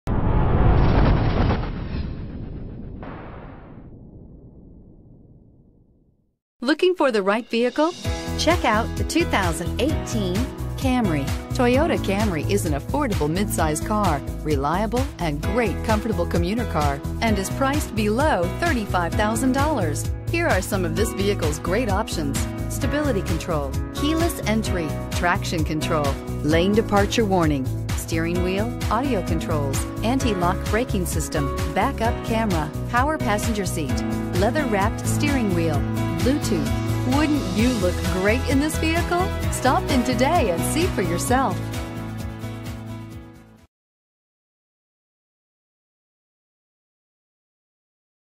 2018 Toyota Camry Metairie LA JL2505
2018 Toyota Camry XSE

Lakeside Toyota
3701 North Causeway

Pick up this Wind Chill Pearl 2018 Toyota Camry, available today at Lakeside Toyota. This could be the one you've been searching for.